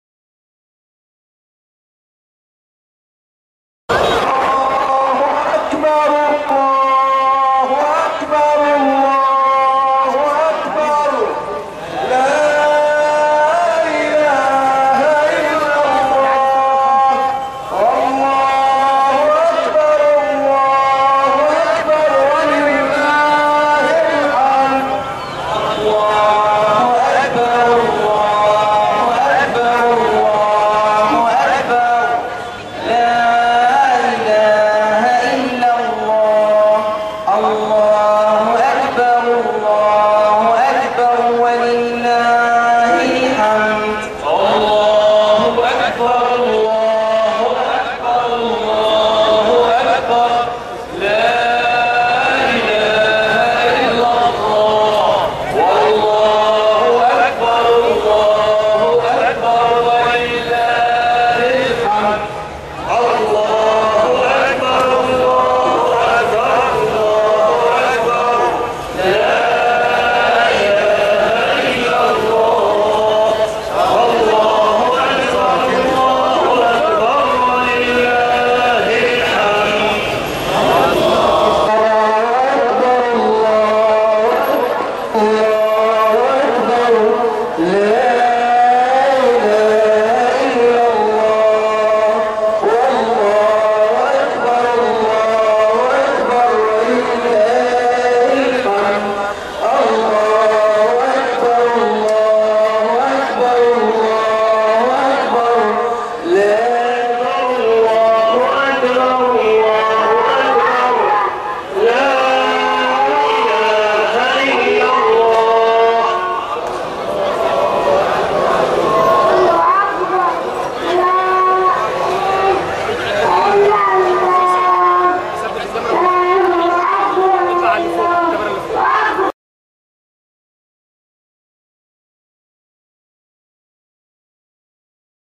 مشاهد للمصلين فى ميدان التحرير يكبرون قبل صلاة العيد
مشاهد للمصلين فى ميدان التحرير يكبرون قبل صلاة عيد الفطر المبارك